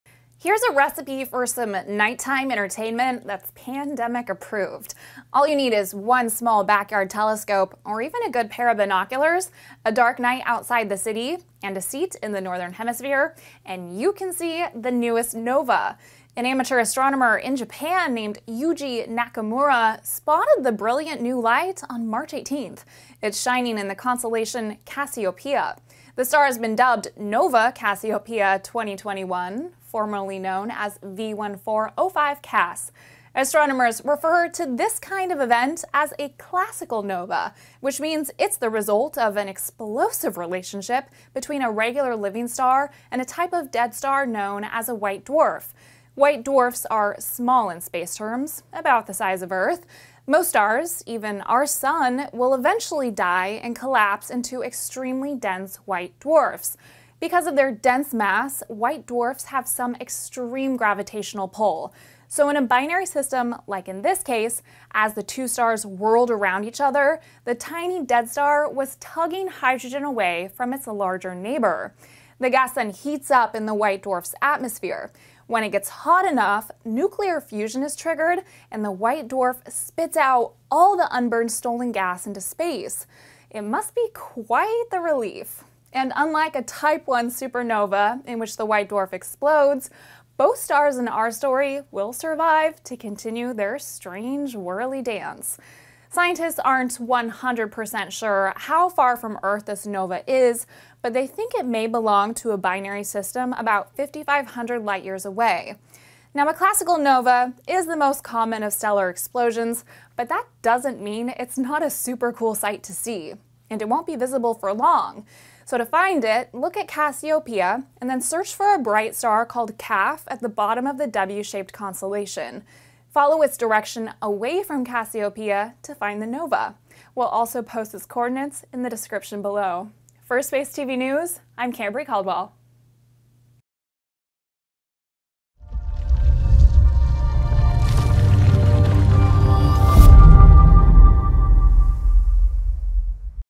Stellar explosion you can see with your own eyes
All you need is a small backyard telescope or a pair of binoculars, dark clear skies away from a city, and you can see this new nova. But it won't be visible for long! Japanese amateur astronomer, Yuji Nakamura discovered V1405 Cas on March 18. Its coordinates are; right ascension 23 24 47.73, declination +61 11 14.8. Happy viewing!

Credits:
NASA's Goddard Space Flight Center/Chris Smith (USRA)
NASA/JPL-Caltech/NASA's Goddard Space Flight Center
European Southern Observatory (ESO)
European Space Agency (ESA)
Northwestern University Academic & Research Technologies/Matthew McCrory
Dom Le Roy
Dmitry Varennikov
Roy U Ivonne

Thumbnail image credit:
NASA's Goddard Space Flight Center/S. Wiessinger

SPACETV.NET 2021